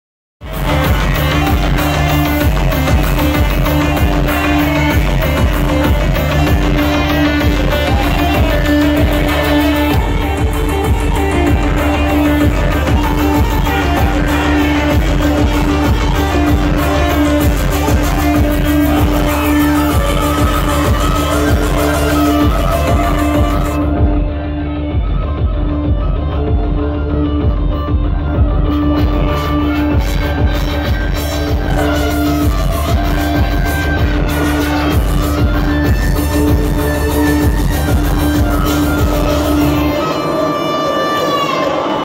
Acid Arab Détonation 28/09/17
Acid Arab au Festival Détonation à Besançon Le jeudi 28 Septembre 2017